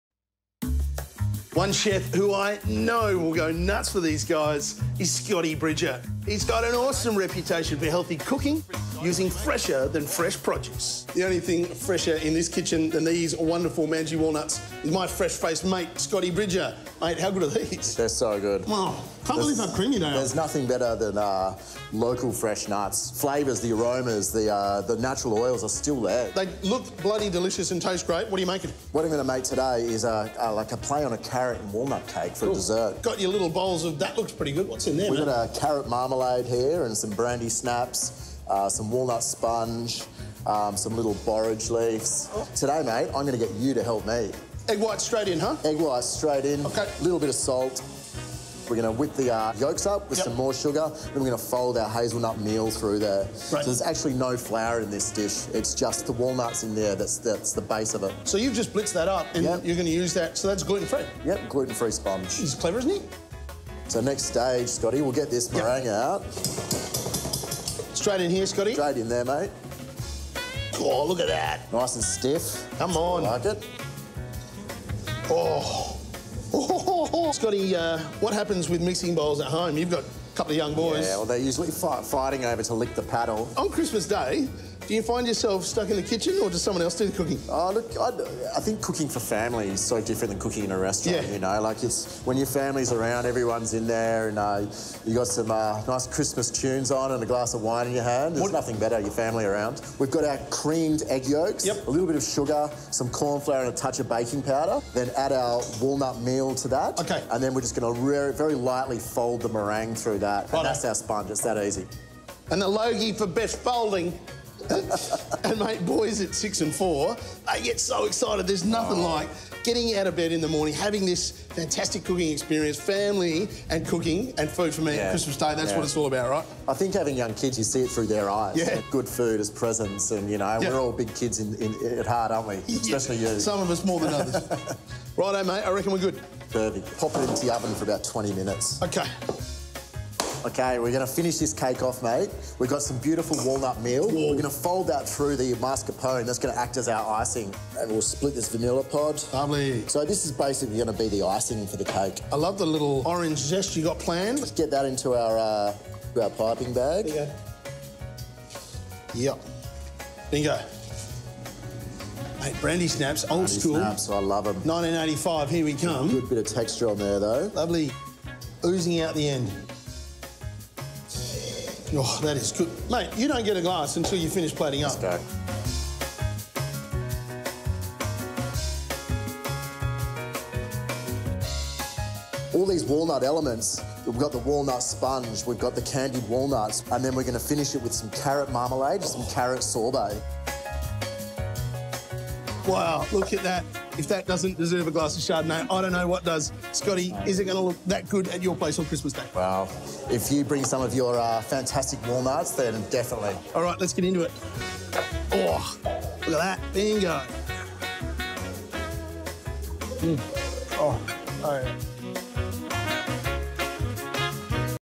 Delish - Scott’s Carrot and Walnut Cake
One chef we know will go nuts for walnuts is Scott Bridger- he’s got an awesome reputation for cooking using fresher than fresh produce. There’s nothing better than a local fresh nuts and the flavours, aromas, the natural oils are just fantastic.